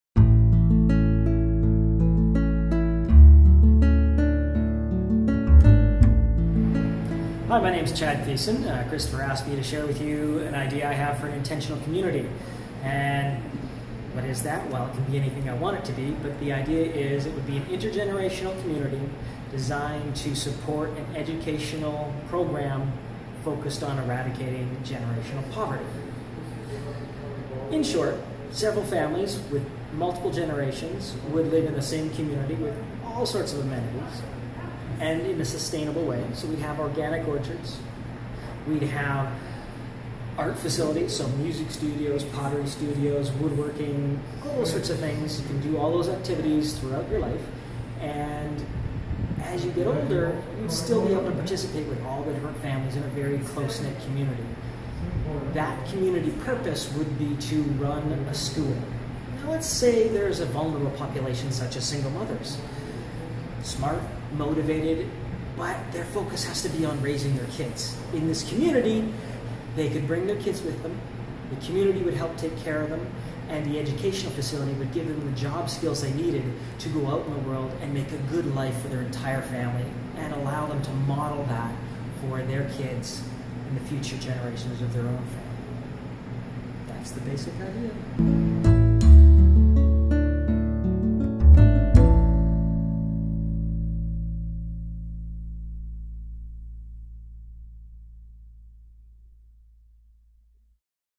Intentional community
One man's intention to bring meaning and support for families to end generational poverty.